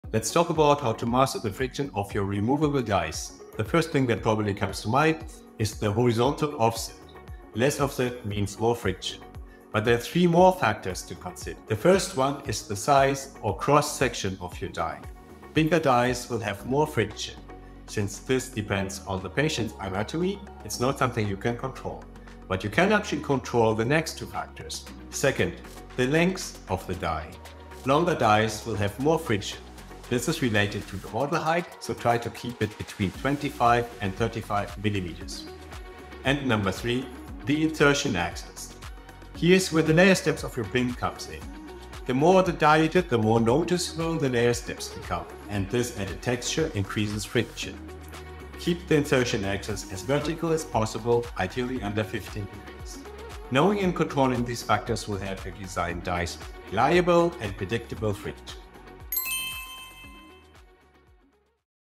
4 Keys to Friction Control in Removable Dies 🔩🦷
Title: 4 Keys to Friction Control in Removable Dies 🔩🦷
Description:
Master removable die friction with 4 key factors:
✅ Horizontal Offset
✅ Cross Section
✅ Die Length
✅ Insertion Angle
Take control of your removable die workflow from CAD to CAM — with secure, predictable results using Precision Model Resin.